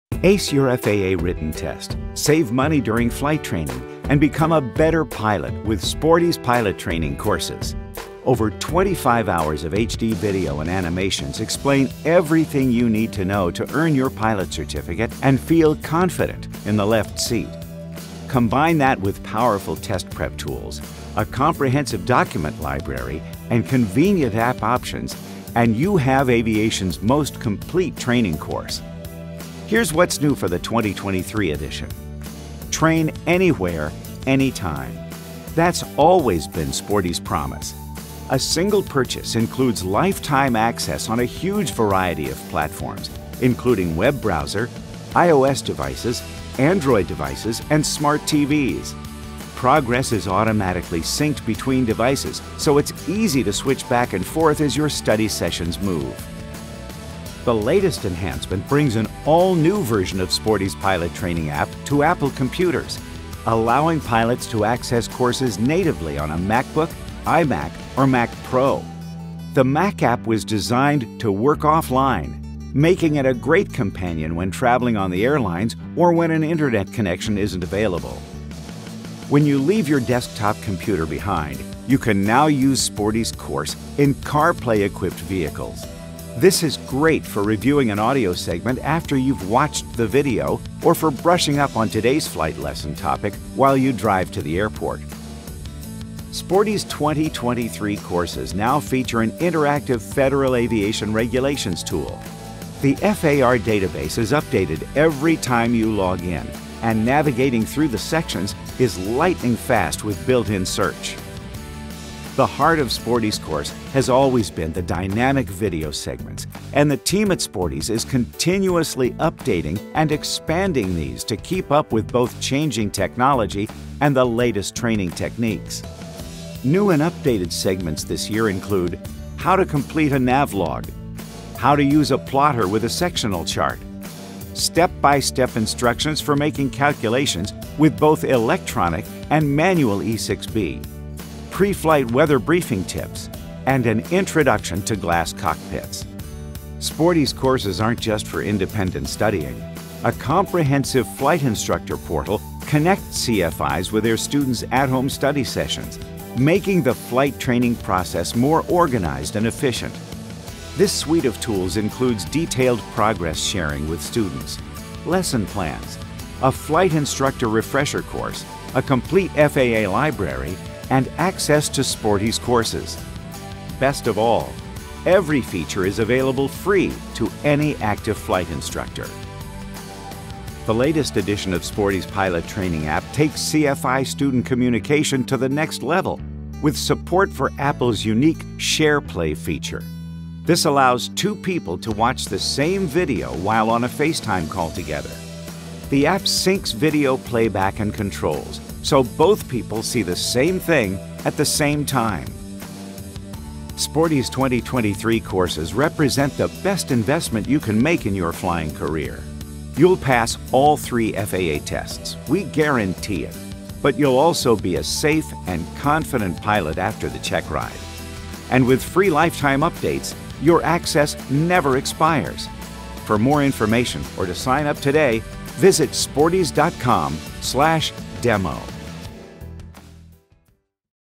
Sporty's 2023 Pilot Training Courses - here's what's new (Mac app, FARs, CarPlay)
The best flight training course just got better!

Ace your FAA written test, save money during flight training, and become a better pilot with Sporty’s Learn to Fly Course. This flexible training platform allows you to create your own training plan and learn at your own pace. Over 15 hours of HD and 4K video and animations explain everything you need to know to earn your Private Pilot certificate and feel confident in the left seat. Combine that with powerful test prep tools, a comprehensive document library, and convenient app options, and you have aviation’s most complete training course.

Now includes TV, Mac and Carplay apps among the 9 included platforms, interactive FAR/AIM tool, PowerStudy test prep tools across all devices, iOS SharePlay, and much more.